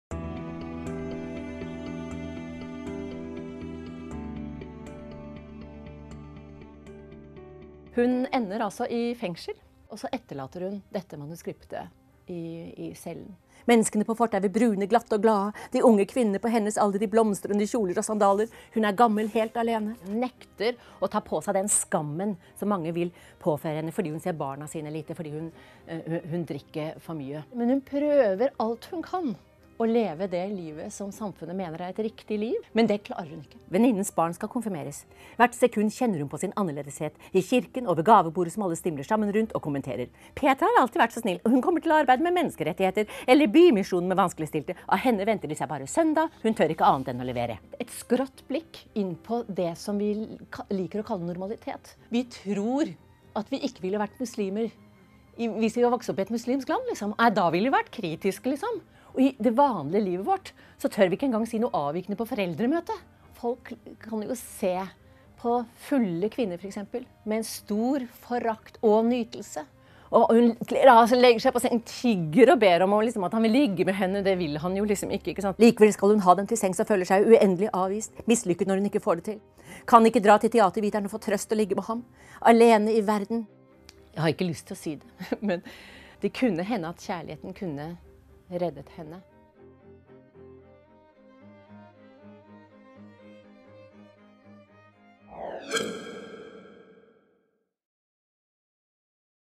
Bokåpner: Tredje person entall
Vigdis Hjorth presenterer boken Tredje person entall. Hulda Kråkefjær kommer fra en lovende familie. Men med Hulda skal det vise seg å gå riktig ille: Hun mislykkes gjentatte ganger i kjærlighetslivet, tilbringer mye tid på brune puber, drikker seg fra sans og samling og gjør håpløse ting. Hulda Kråkefjær lever rett og slett et destruktivt liv. Hvorfor har det gått så dårlig med Hulda? Hvordan kunne det gått annerledes? I hvilken grad kan man styre skjebnen?